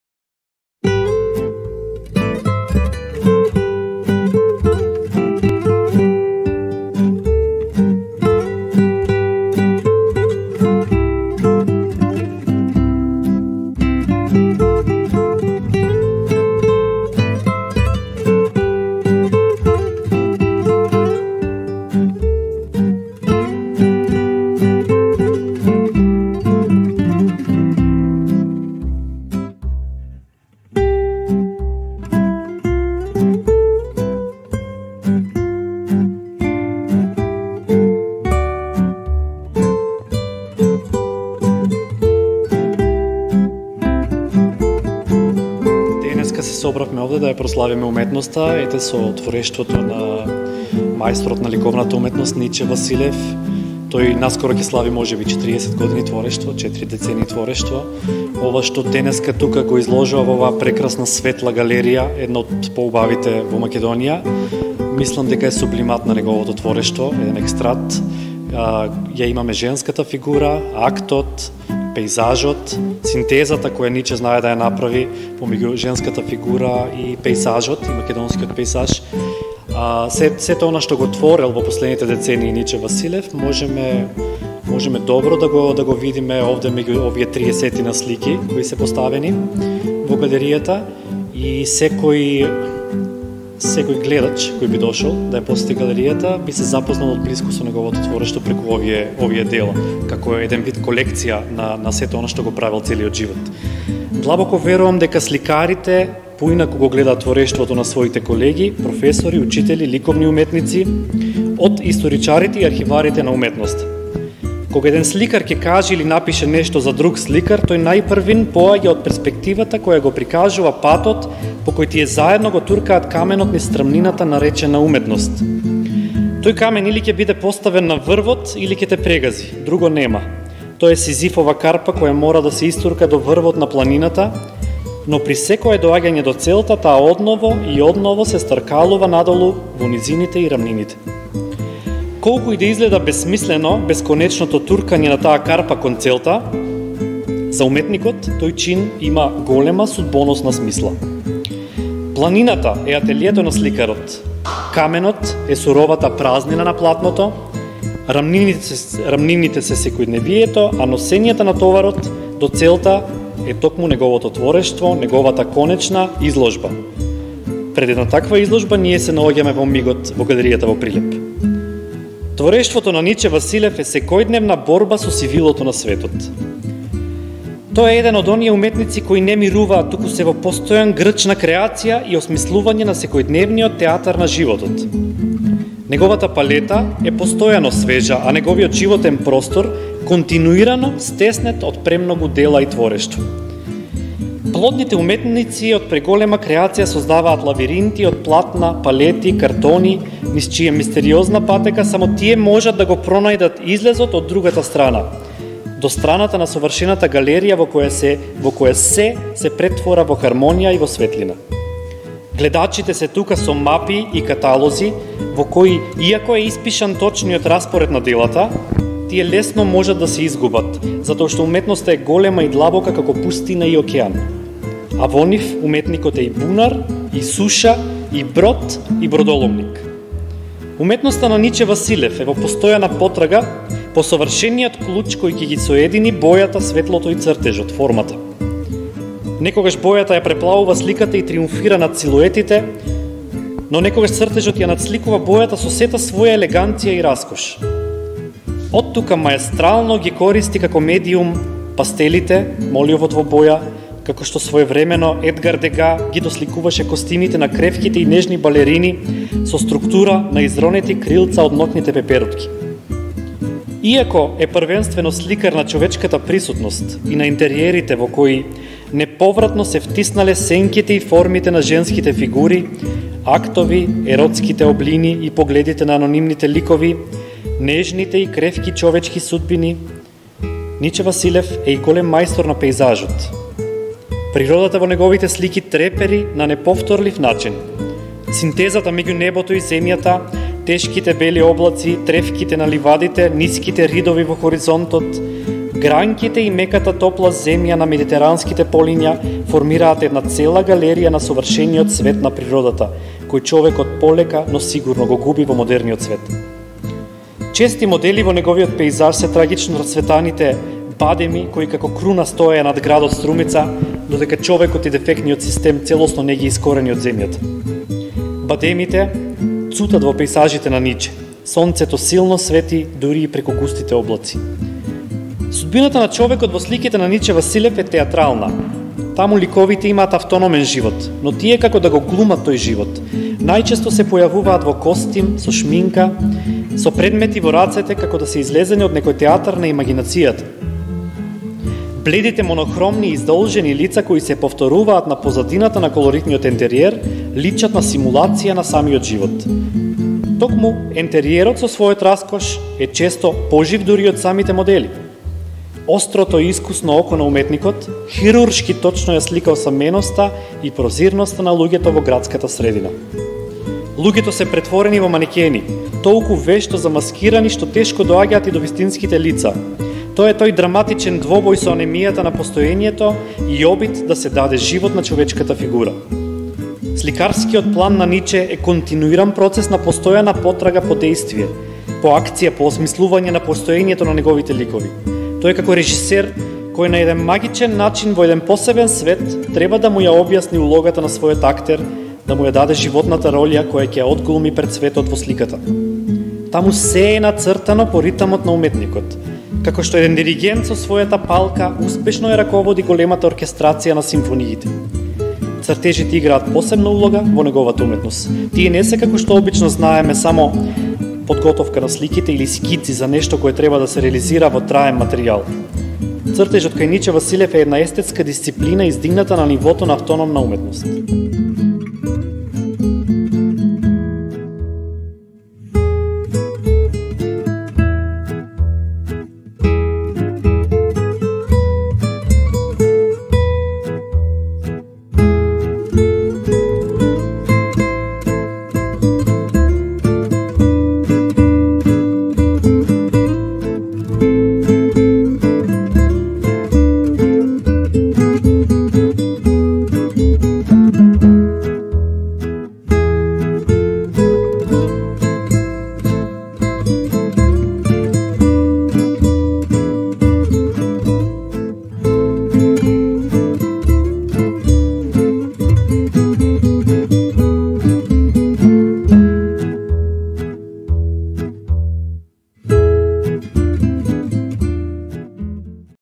Изложба на слики од Ниче Василев
Во Ликовната галерија од ЦК„Марко Цепенков”отворена изложба на слики од уметникот Ниче Василев, кој пред прилепчани се претставува со 22 дела. Изложбата поддржана од Министерството за култура ќе биде поставена до 26 мај 2021.